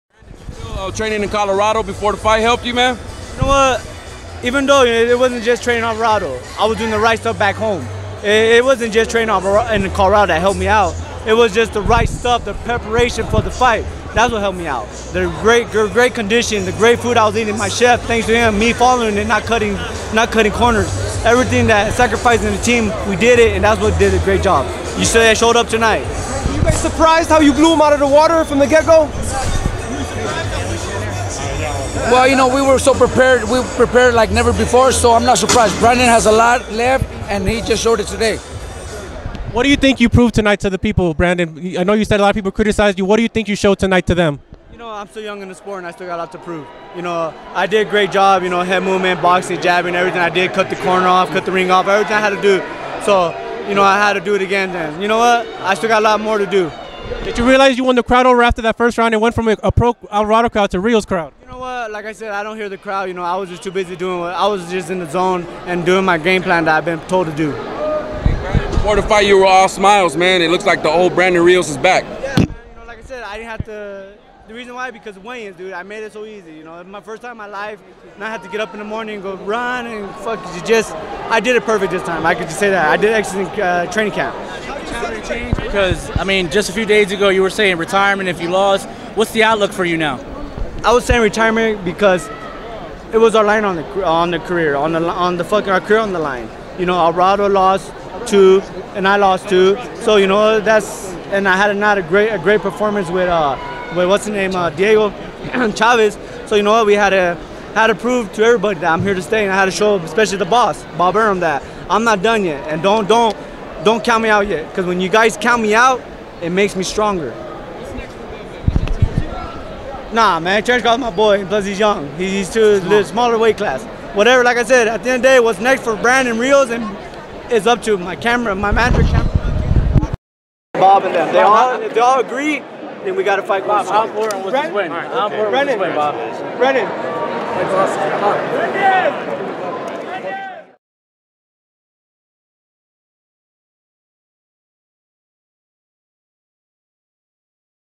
BRANDON RIOS SAYS HE PROVED A LOT IN WIN OVER ALVARADO: "I WAS JUST IN THE ZONE"
"I did a great job...head movement, boxing, jabbing, everything I did, cut the corner off, cut the ring off...I was just in the zone and doing my game plan that I've been told to do...don't count me out yet because when you guys count me out, it makes me stronger," stated former world champion Brandon Rios, who spoke more about his dominant victory over Mike Alvarado. Check out what he had to say about his performance and much more!